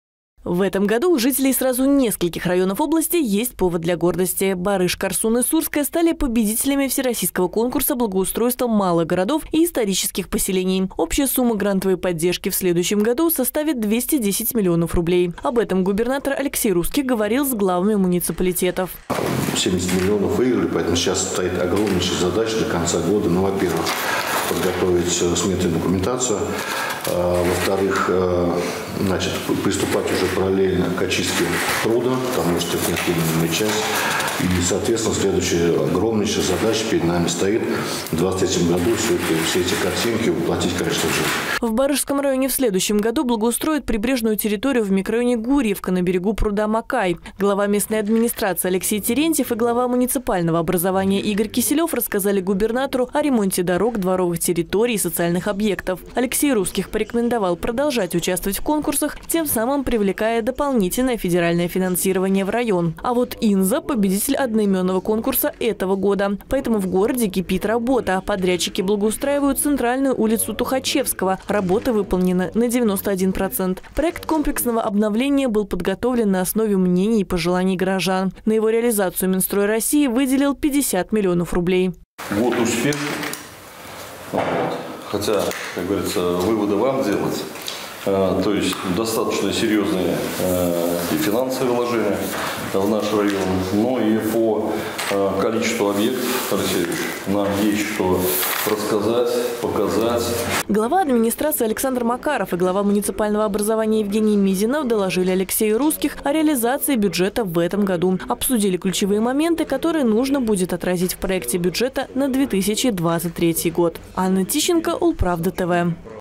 В Инзе – центр, в Барыше – на берегу. Как благоустраивают районы Ульяновской области
Что уже сделано, что предстоит и что не удалось. Главы муниципалитетов Ульяновской области отчитываются перед губернатором о проделанной работе в вверенных им районах. Планируют и бюджет на следующий год. В нашем материале пойдет речь о Барышском и Инзенском районах.